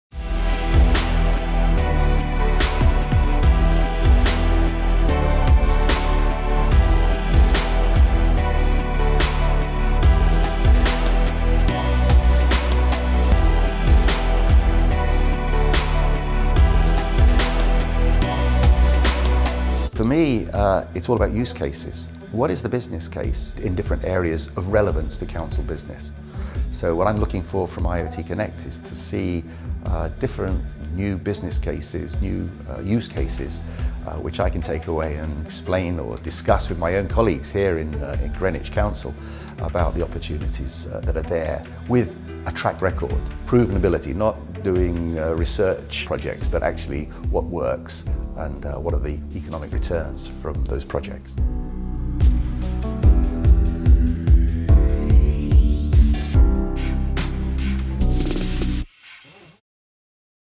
Smart IoT Connect 2019: Allan Mayo, Digital Greenwich
Allan Mayo., Smart City Strategist, Digital Greenwich, shares why he's attending Smart IoT Connect, September 10, 2019 (London)